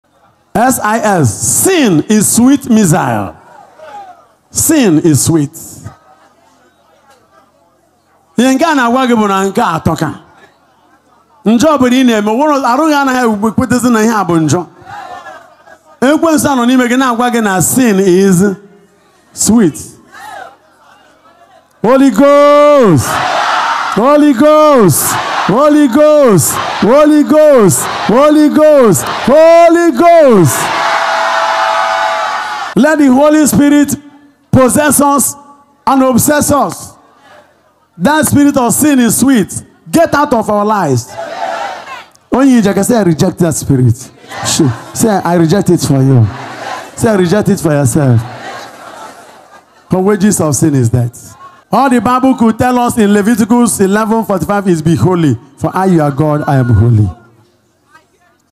Rev Fr Ejike Mbaka: "Be delivered from S.I.S, Sin is Sweet Syndrome, and walk in righteousness.
Rev Fr Ejike Mbaka: "Be delivered from S.I.S, Sin is Sweet Syndrome, and walk in righteousness. Amen. adorationministryenugunigeria revfrejikembaka Amen"